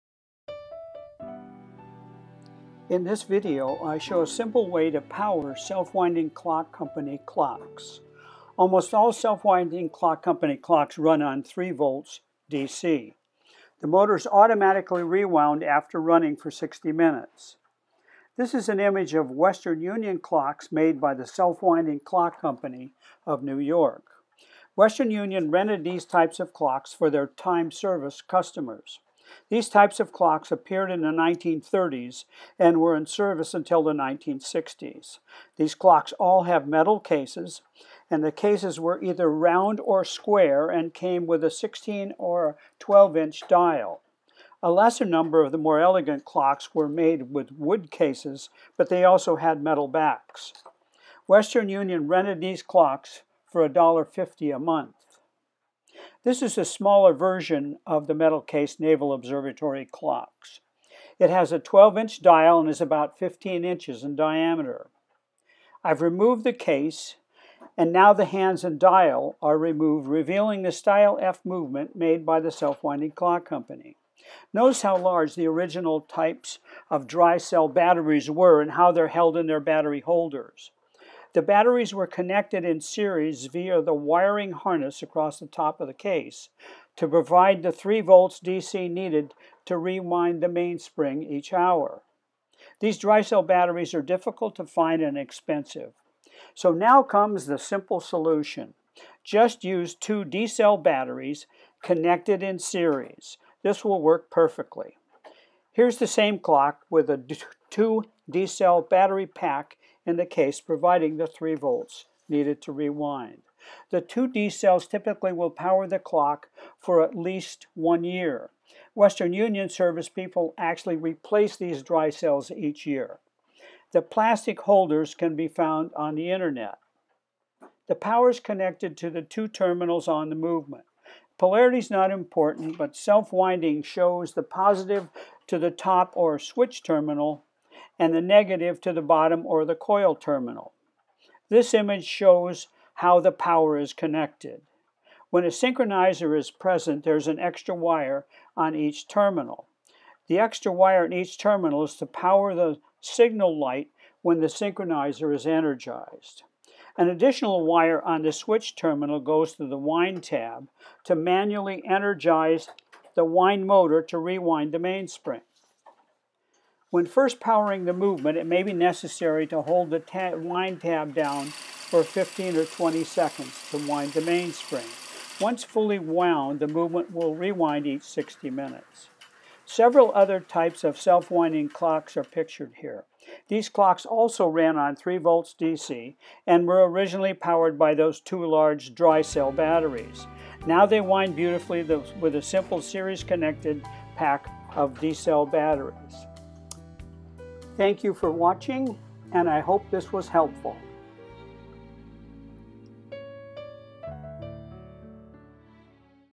How to power a Self Winding Clock Company clock
This video shows how to connect the 3 VDC power supply to the Self Winding Clock Company style "F" movement. A very simple power supply can be used.